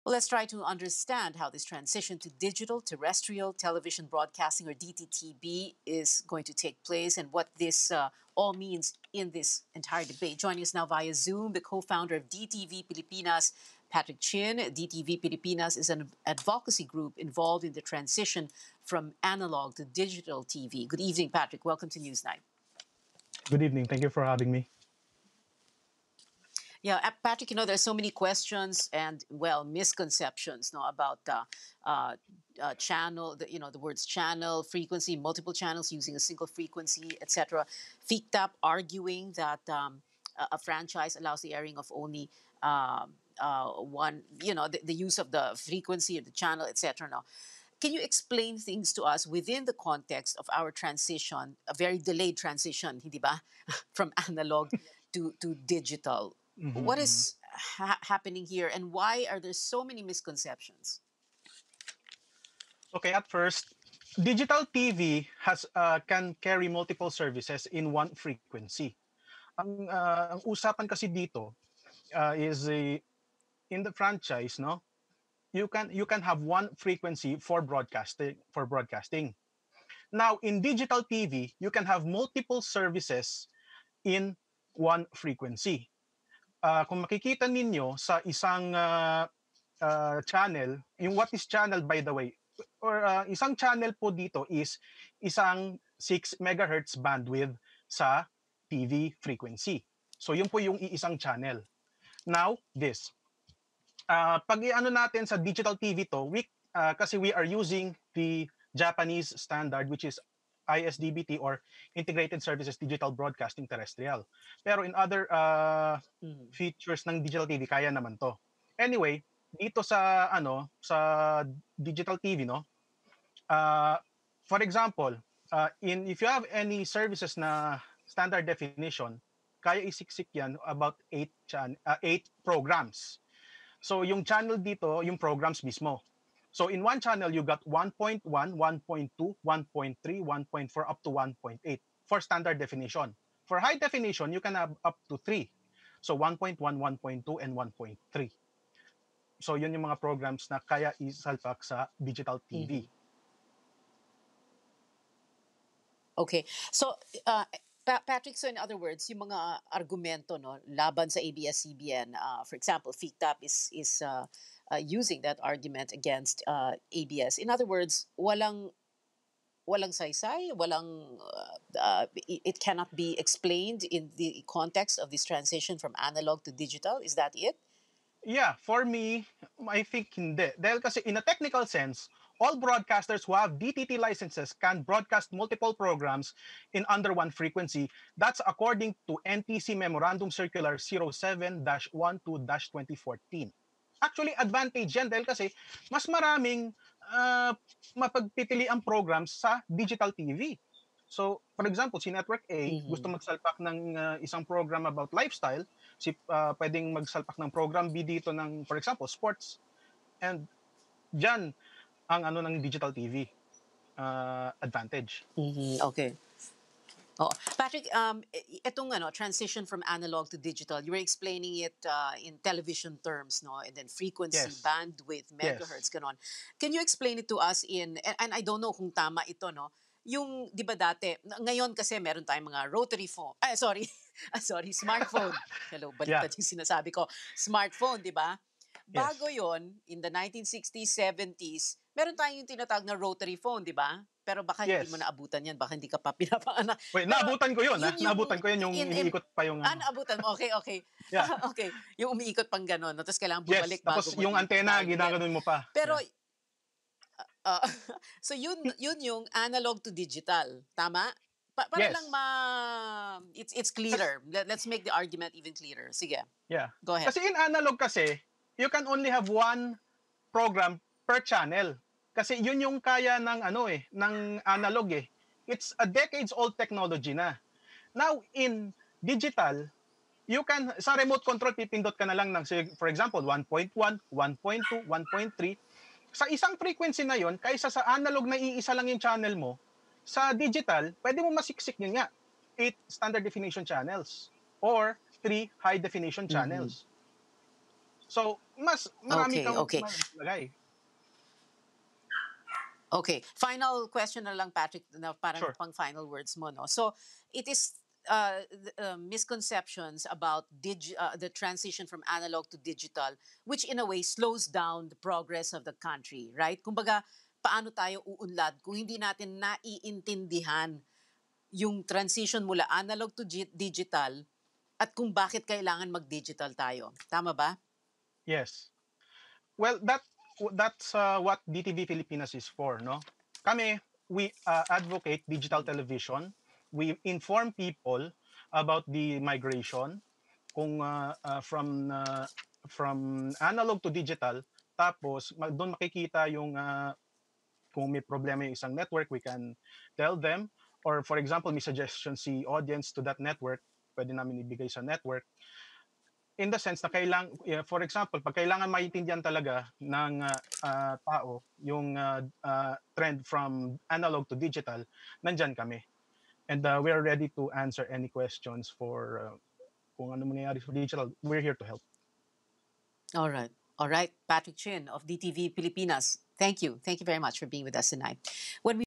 One franchise, one channel?
Let's try to understand how this transition to Digital Terrestrial Television Broadcasting or DTTB.
We are joined by the Co-Founder of DTV Pilipinas, Patrick Chin.
DTV Pilipinas is an advocacy group involved in the transition from analog to digital TV.